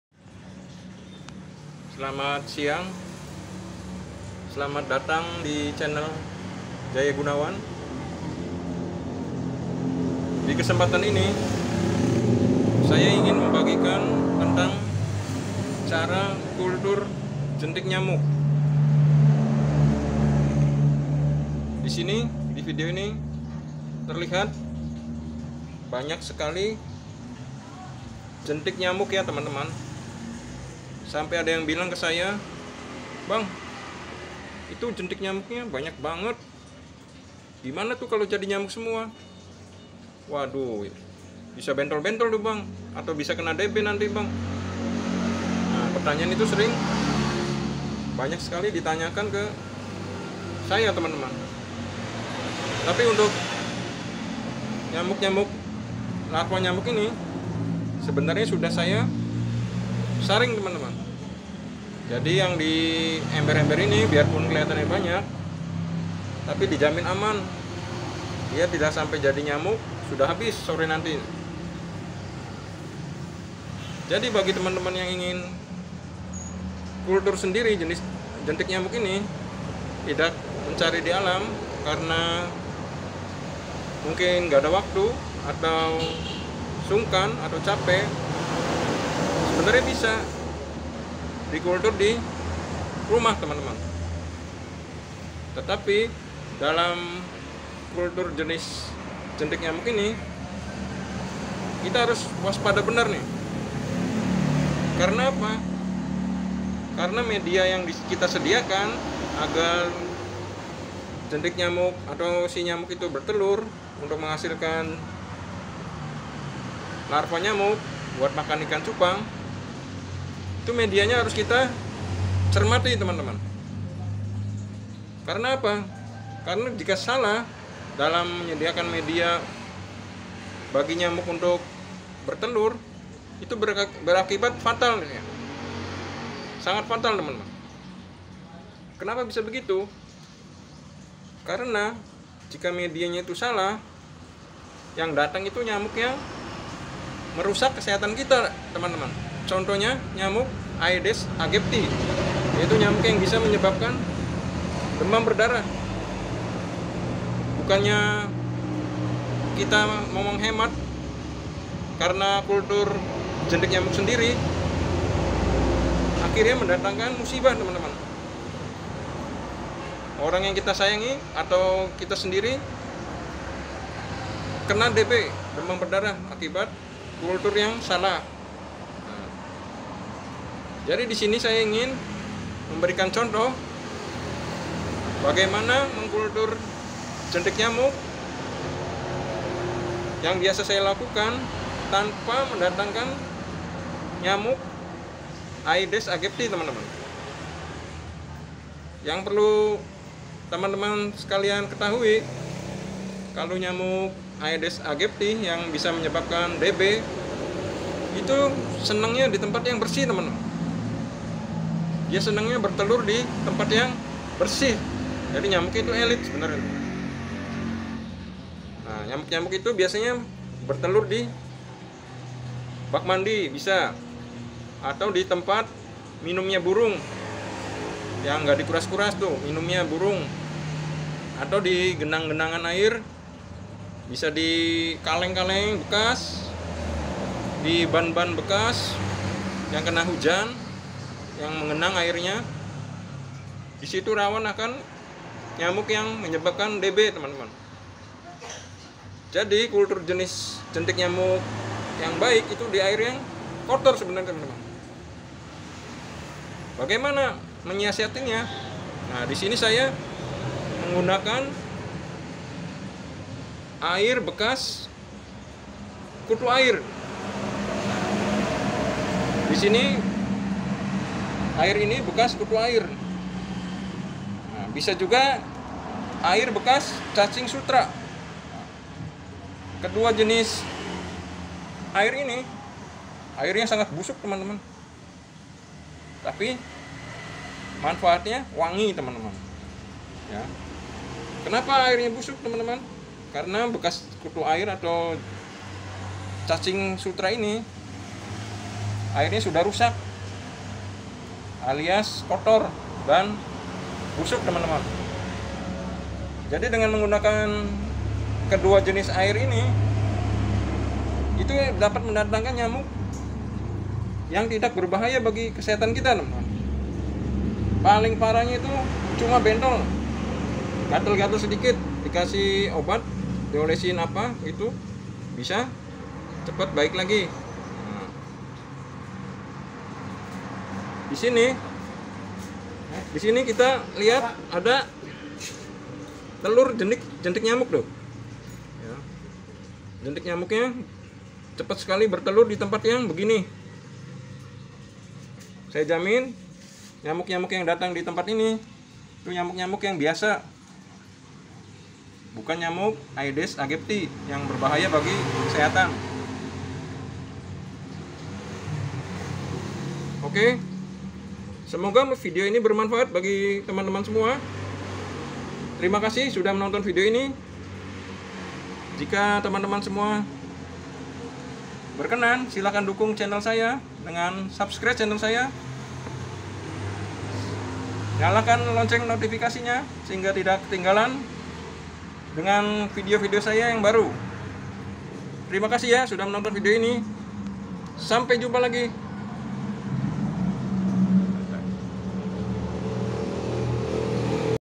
kultur jentik nyamuk modal air bekas
penghobi ikan cupang harus tahu cara kultur jentik nyamuk, @124bettafish membagikan  pengalaman dalam kultur jentik nyamuk.
ada beberapa hal yang perlu dicermati untuk kultur jentik nyamuk.
semoga bermanfaat
terimakasih